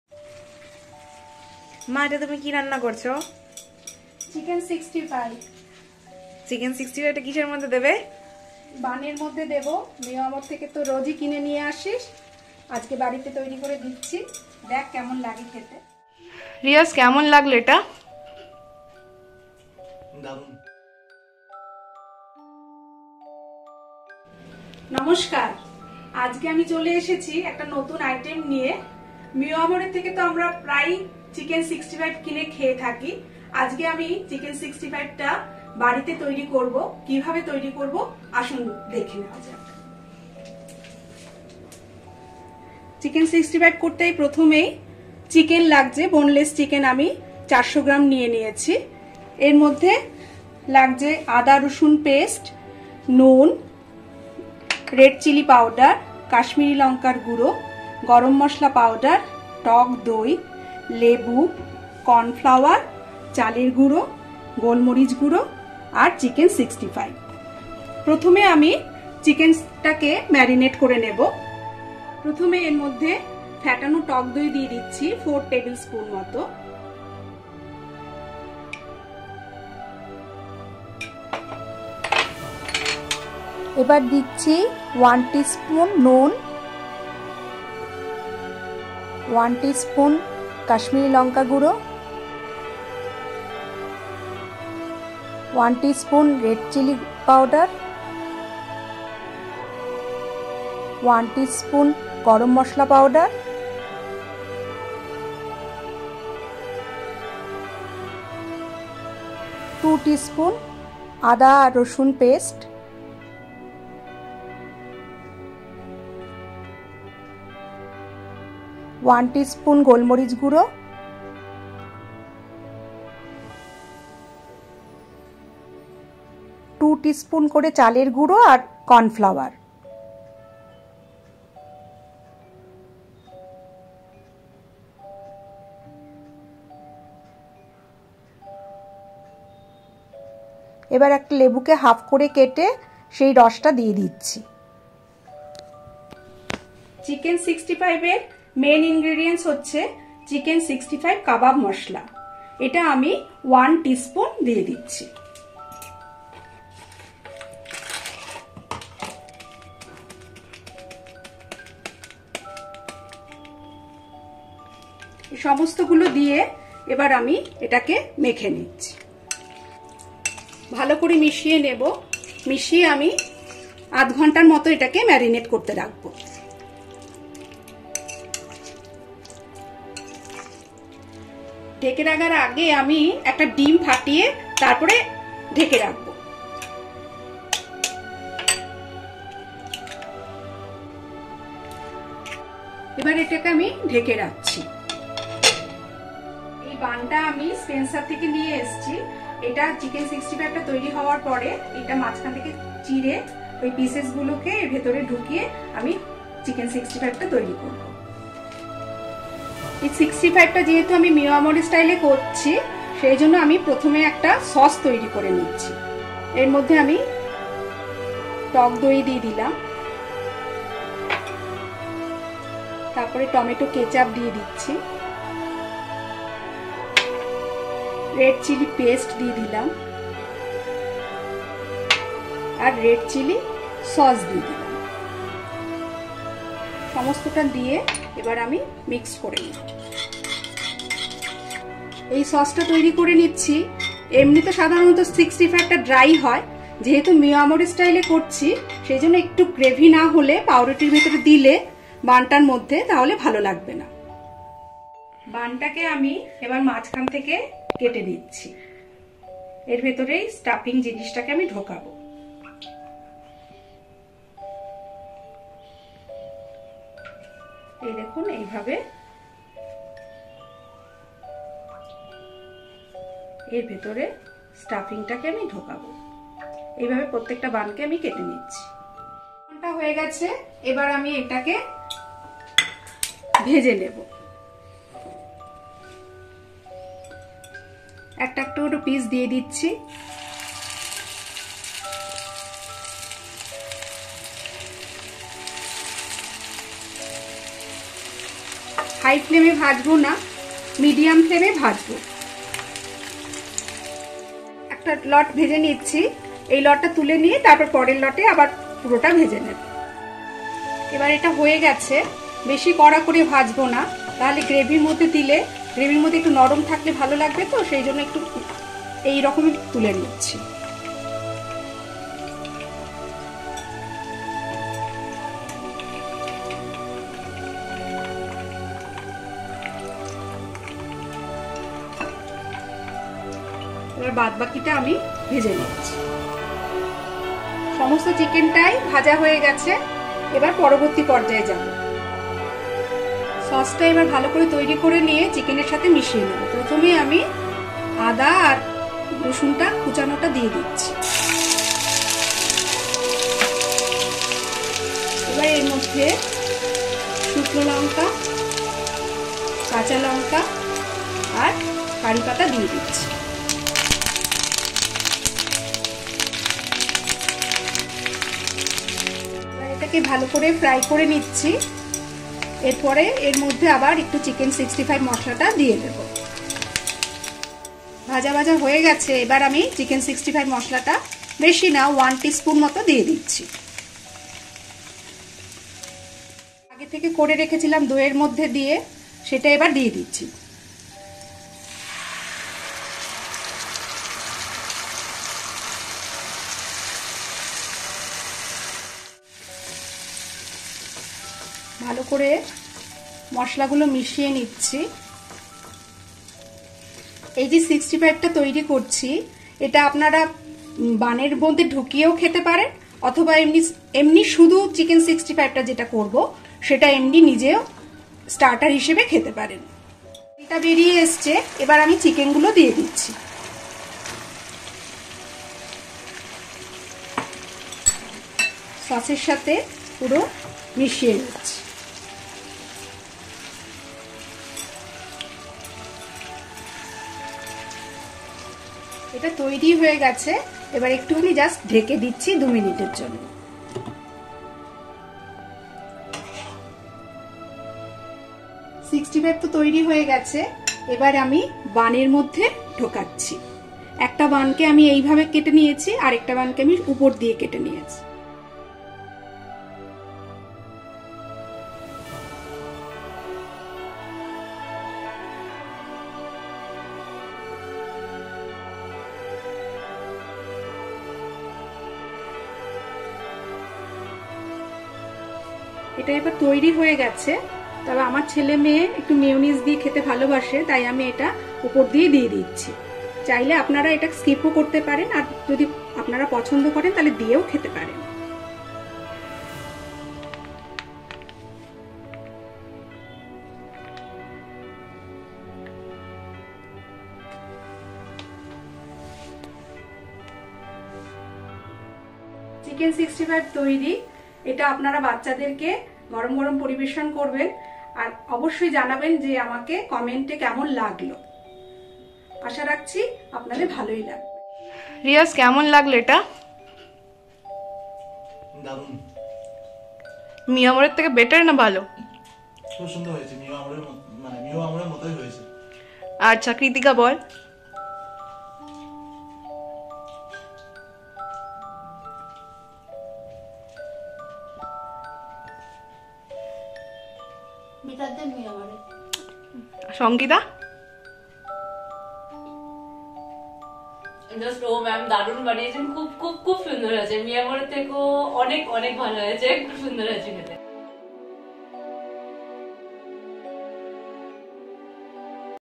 Homemade Mio Amore Style Chicken 65 | Chicken 65 with gravy | Chicken 65 recipe | Mio Amore Special
|| Homemade Mio Amore style Chicken 65 ||

Ingredients:-

For marination
1. Boneless chicken - 400 gms
2. Lemon juice - Half slice
3. Red chilli powder-  1tsp
4. Kashmiri Red chilli powder- 1tsp
5. Ginger garlic paste- 2 tsp
6. Salt -1 tsp
7. Black pepper powder- 1tsp
8. Curd - 4 tsp
9. Cornflour- 2 tsp
10. Rice flour - 2tsp
11. Egg - 1
12. Garam masala powder - 1tsp
13. Chicken 65 kebab masala - 1tsp

For making sauce -
1. Tomato ketchup - 2 tbsp
2. Red chilli paste - 2 tsp
3. Curd- 2tbsp
4. Red chilli sauce- 2tbsp

Now other ingredients

1. Buns - 4
2. Refined oil - 5tbsp
3. Curry leaves - 20 -30
4. Red chillies - 2
5. Green chillies - 2
6. Chopped ginger &garlic - 2tsp

Procedure:-

1. At first marinate the chicken with the ingredients as mentioned above for 2 hours.

2. Then after 2 hours heat refined oil and fry the chicken pieces on medium flame.

3. When chicken gets fried properly then take them out and keep them aside

4. After that make a sauce with the ingredients as mentioned above

5. Now heat oil again and add chopped ginger and garlic ,then curry leaves, two green chillies, two red chillies and chicken 65 masala and fry them for few minutes.

6. After that add the sauce and until oil comes out stir it well.

7. Then add the fried chicken pieces and mix them with the sauce and then cover it on low flame for 2 mins.

8. After 2 mins remove the lid and fill the chicken 65 inside the hotdog buns and enjoy with onion slice or mayonise or tomato sauce.